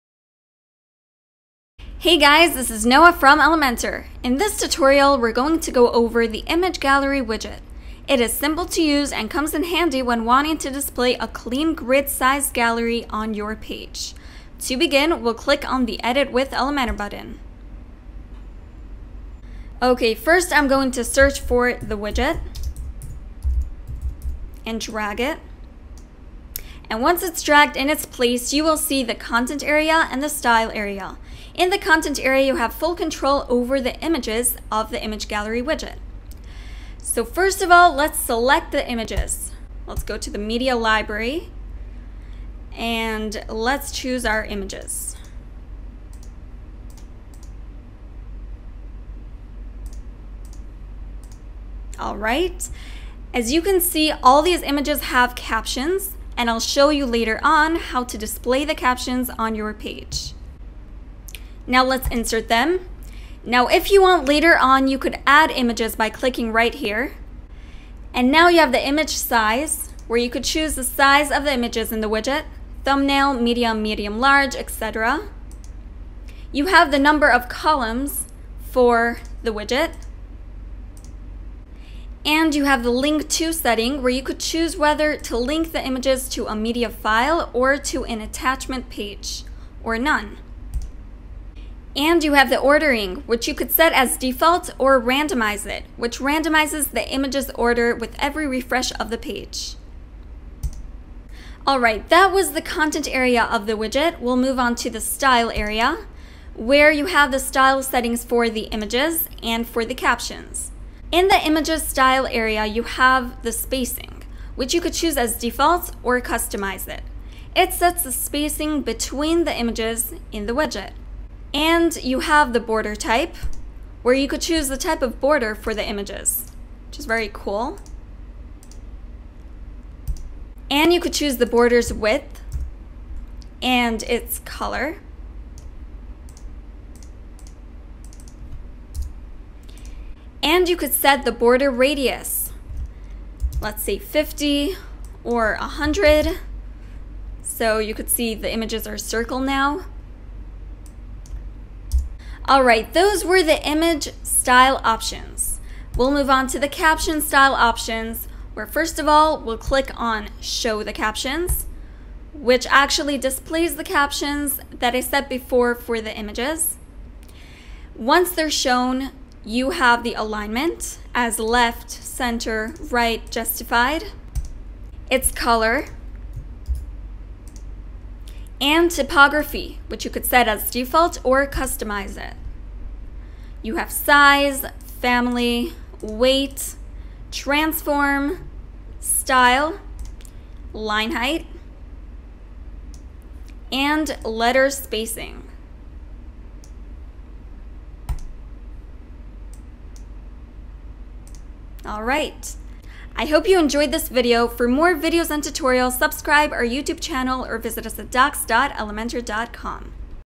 How to Use Image Gallery Widget on Elementor Page Builder Plugin
Easily add images galleries to WordPress with Elementor.

If you are looking for the best, and easiest plugin to add an image gallery to your WordPress website, then look no further, because it's actually easier and less of a burden on your WordPress to use your Elementor page builder plugin for that. It is simple to use and comes in handy when you want to display a clean grid like gallery on your page.

First, we search for the widget called 'Image Gallery'.
Once it is dragged in its place you will see the style area and the content area.
Let's select the images, and insert them.
I have the image size setting, to control the size of the images.
The number of columns, and the 'link to' setting.
We can choose to randomize the order of the images.
I can also change the spacing, and add a border or set a border radius.